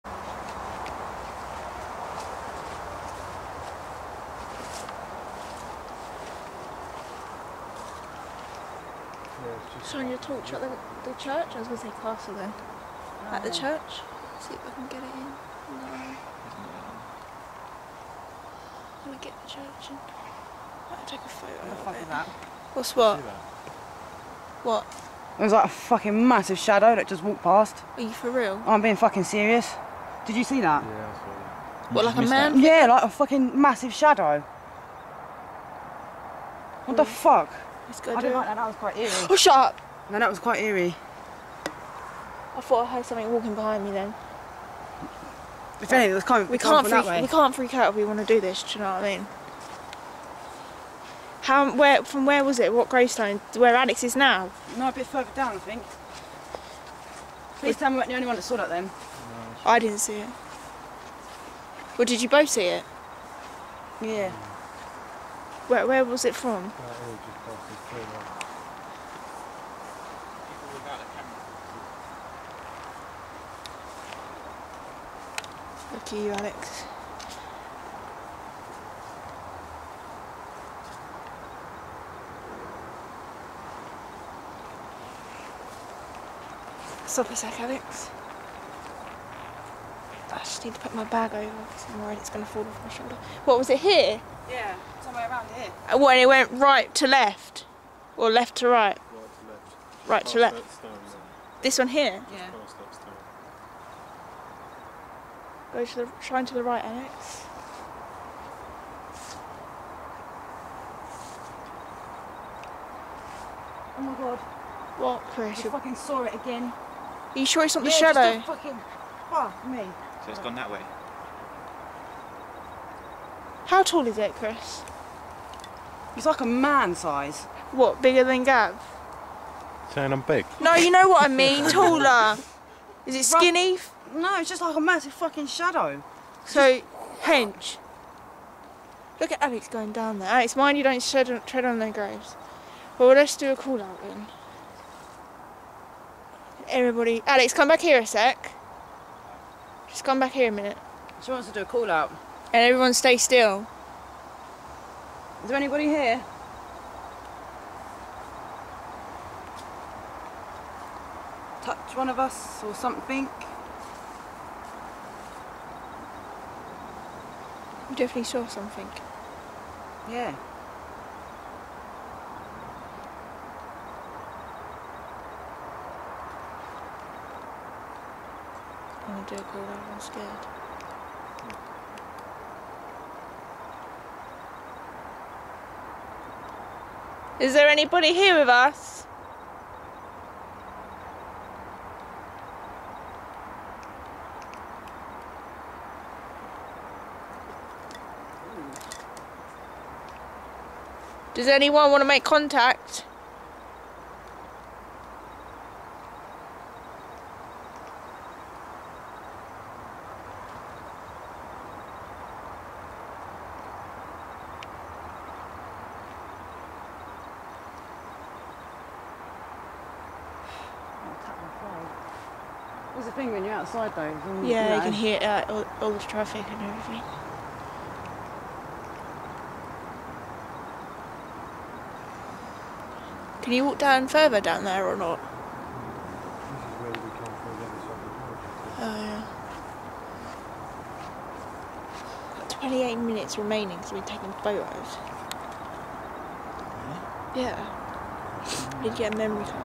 Worth Church Continued
Heres the second part of Worth Church. Gav and my sister both said they saw a dark shadow figure quickly go passed and my sister saw it twice. We all got a eerie feeling from the back off the graves. I decided to do a call out but got no response. Did you guys hear anything strange??